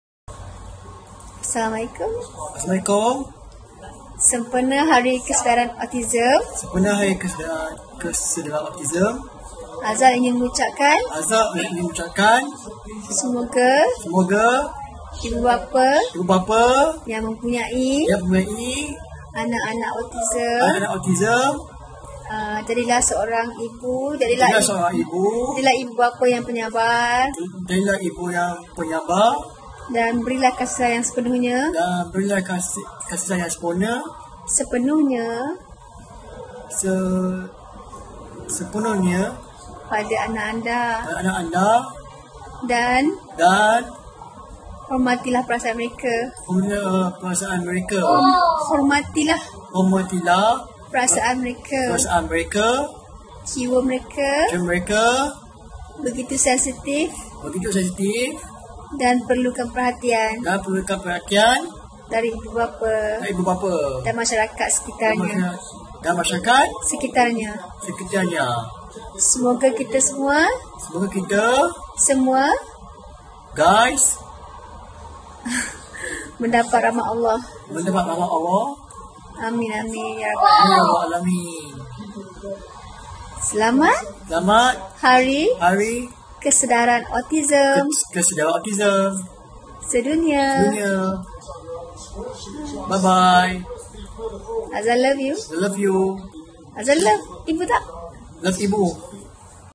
Ucapan Azal Anak Zarina Zainuddin Sempena Hari Kesedaran Autisme Sedunia 2017
Semoga semua ibu bapa yang dikurniakan anak Autisme akan terus cekal dan tabah.